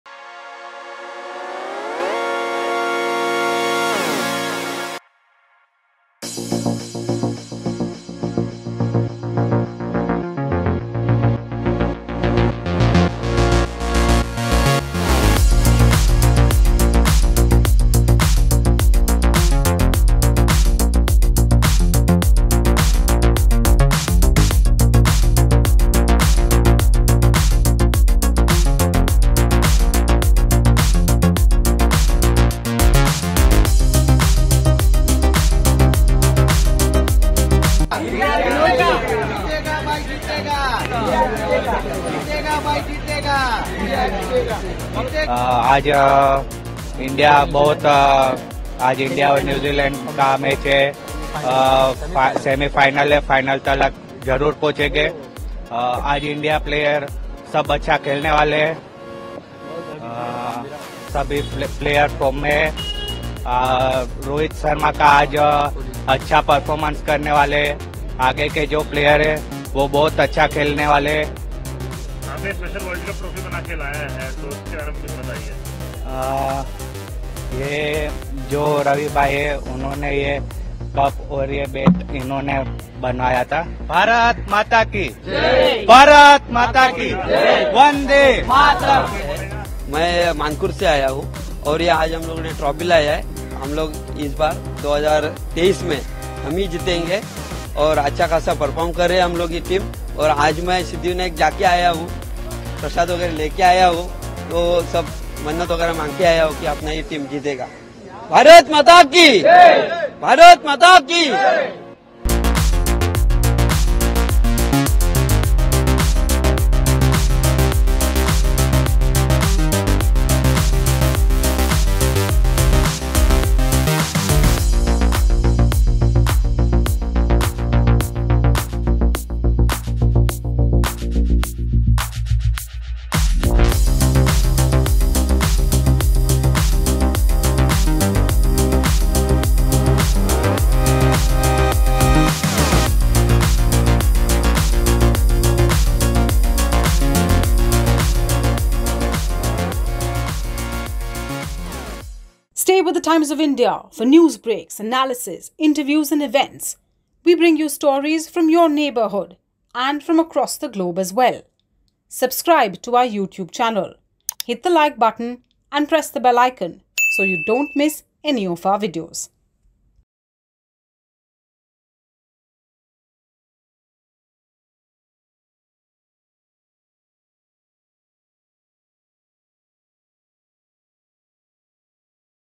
ICC World Cup 2023: India or New Zealand, Who will triumph at Wankhede? Fans REACT!
India faces New Zealand in the ICC World Cup's first semifinal after securing an unbeaten record in the league stage. The anticipation is palpable as fans, buoyed by India's flawless performance so far, fervently hope for a continuation of this winning streak against the Kiwis. The enthusiasm reaches a crescendo as a massive crowd converges at Mumbai's Wankhede Stadium, eager to witness the clash of cricket titans. With high hopes and bated breath, spectators anticipate a thrilling encounter, yearning for India to triumph and secure a spot in the coveted World Cup final. The stage is set for a cricketing spectacle at Wankhede.